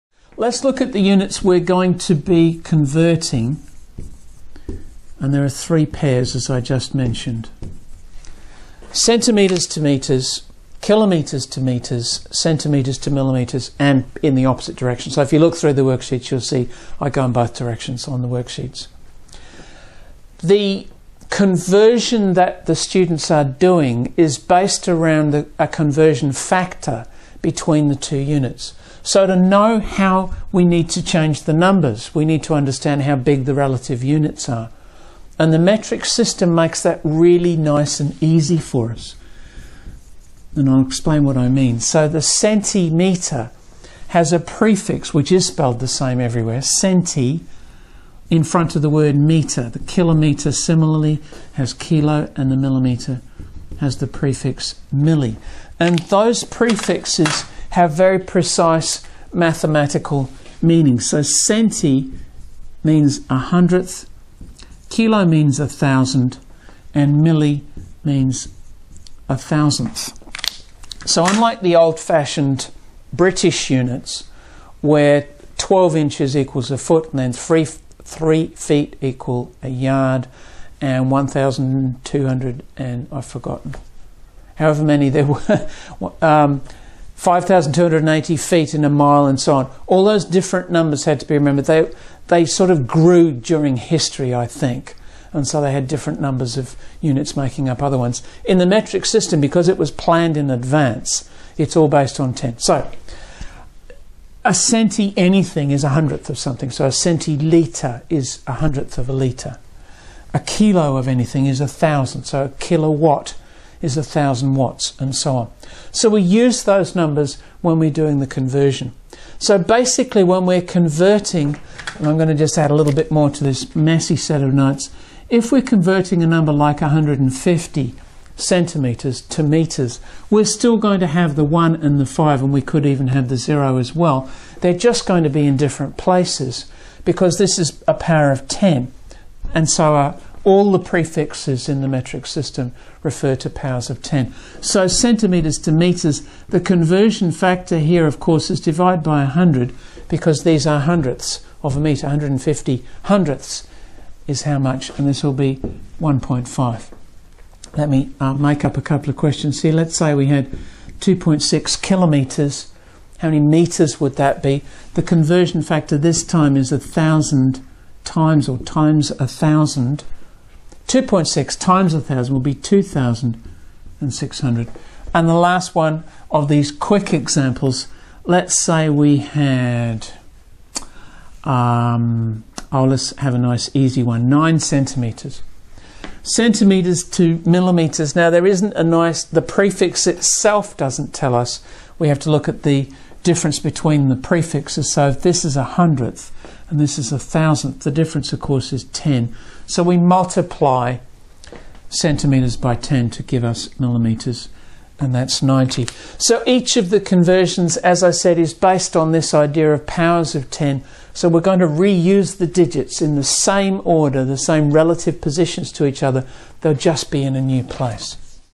Teaching Conversions of Metric Length (short version)
The metric (SI) system of units of measurement uses powers of ten for the various units of different magnitudes. This makes converting between the units really straight forward.

Talk to students about moving the digits in a number, rather than moving the decimal point, when converting. For example:

23.6 m = 2360 cm (move each digit two places to the left)